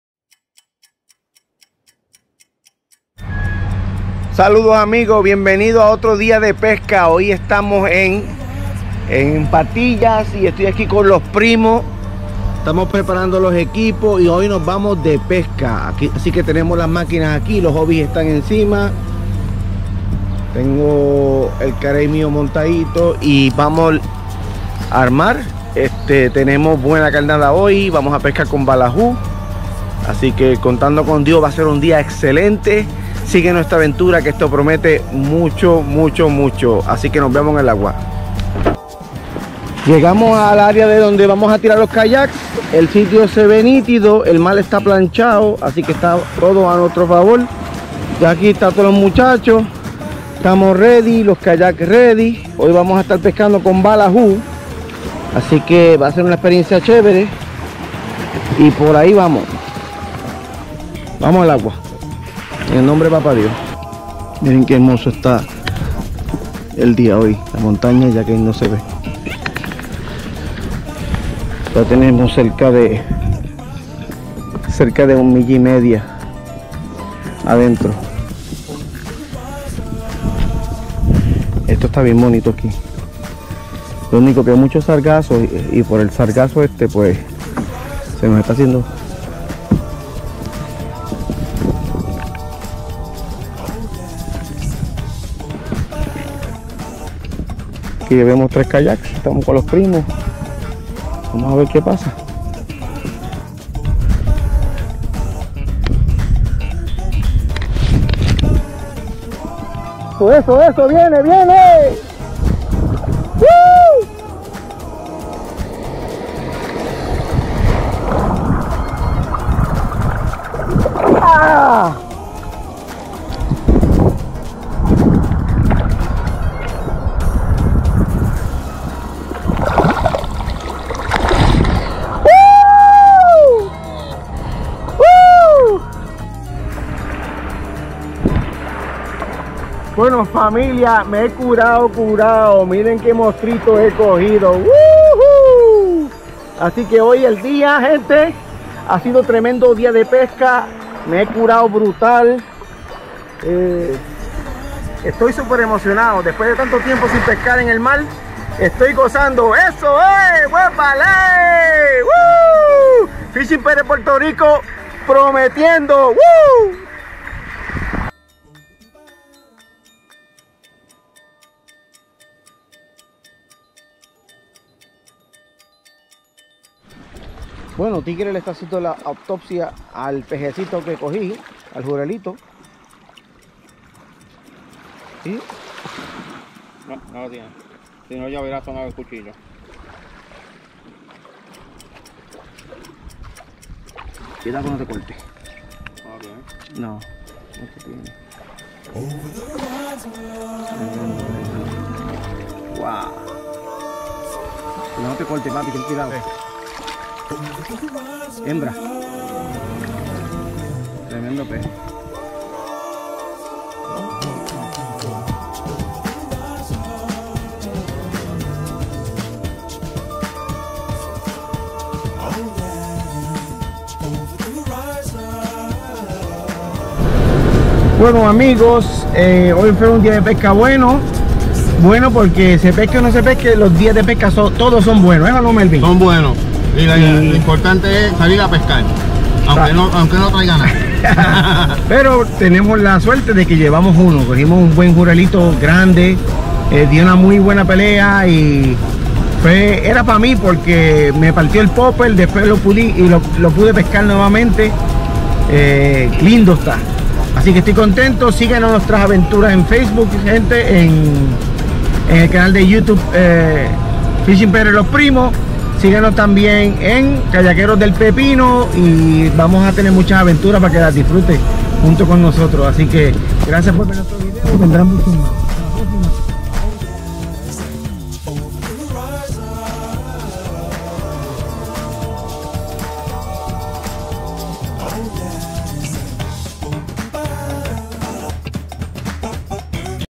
Aventura kayakpesca en Patillas PR, día 2nd con los Primos.
kayakpesca con los Primos en Patillas PR, 2do día, nos curamos!!!